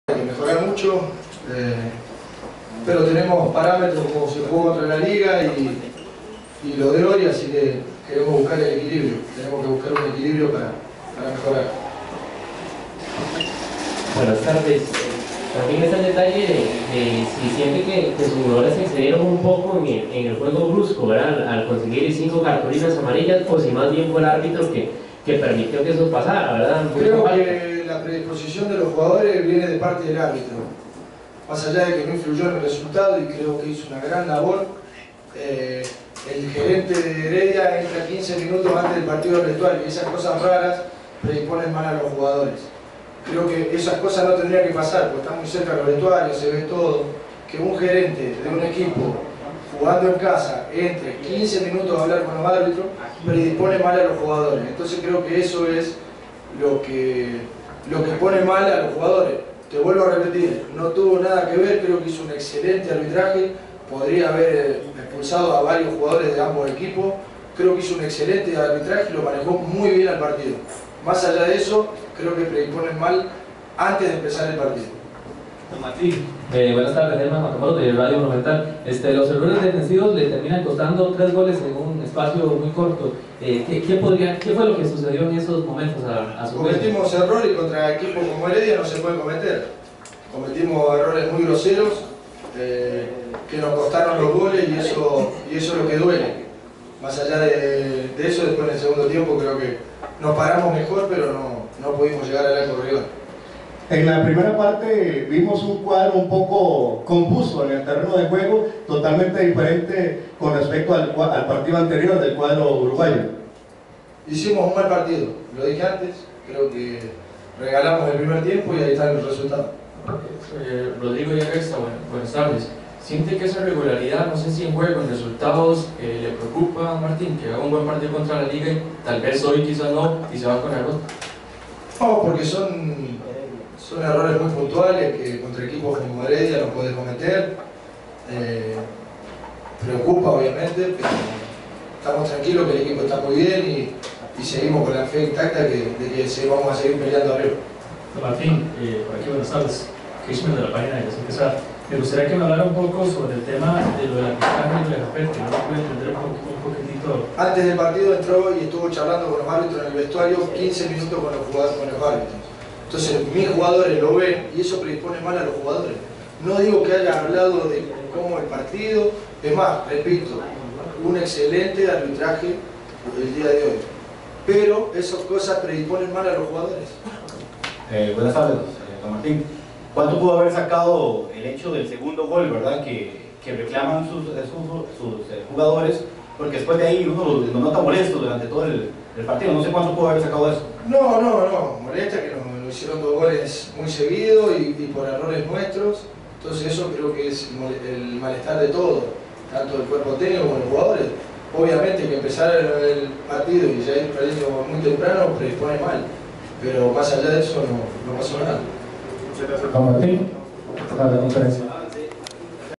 Declaraciones Martín Cardetti (Juego Herediano - Uruguay)
"Los camerinos están muy cerca y se ve todo; que un gerente de un equipo, jugando en casa, entre 15 minutos al vestuario... eso pone mal a los jugadores"